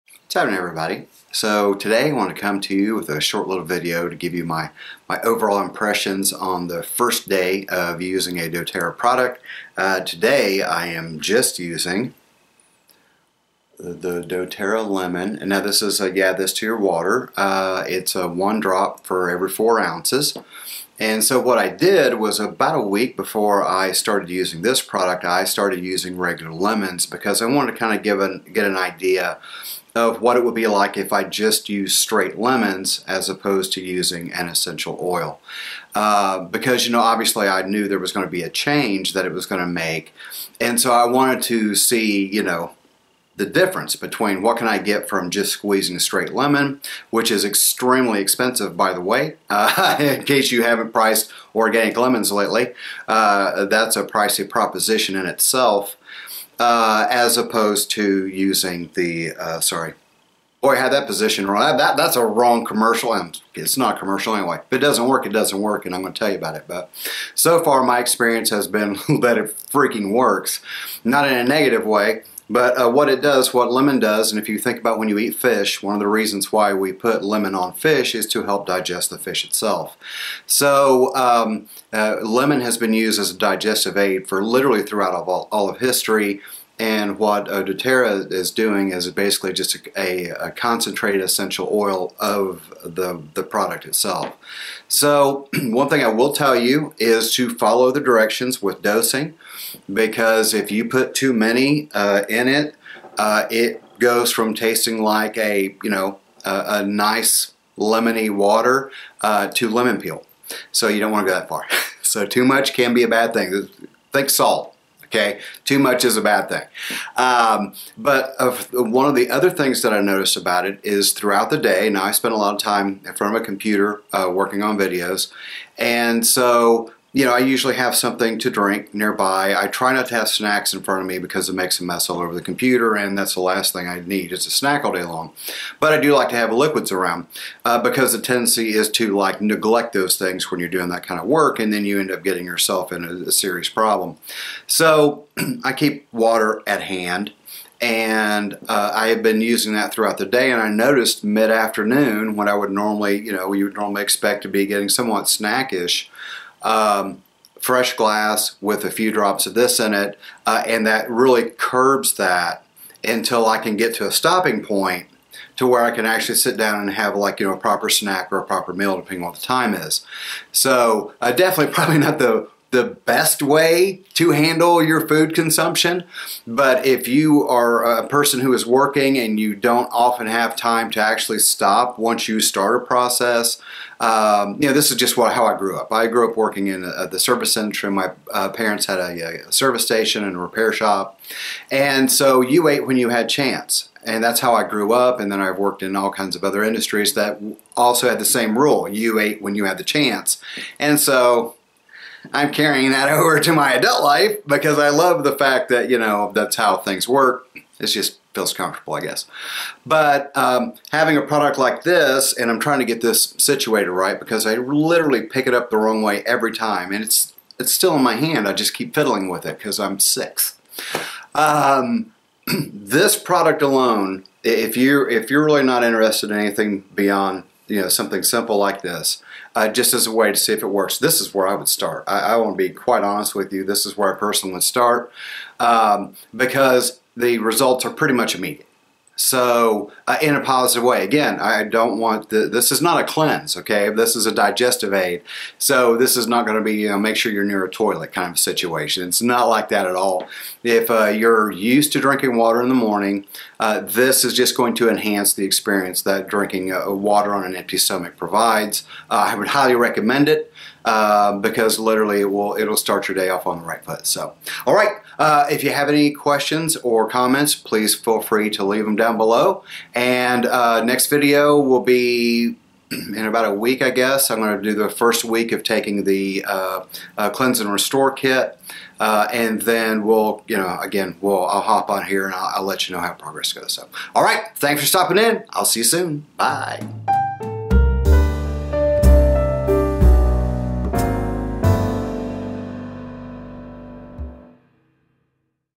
My doTerra Journey Pt 2: Lemon
Join me today as I go over my first day using Lemon from doTerra, an essential oil derived from, well, lemons. I'll go over how it stacks up next to a regular lemon, and any unusual benefits that might come from Lemon over, uh, lemons. Slightly confusing, I know, but I'll explain everything in the video!

If you're interested in finding out more, feel free to check out my link on their products!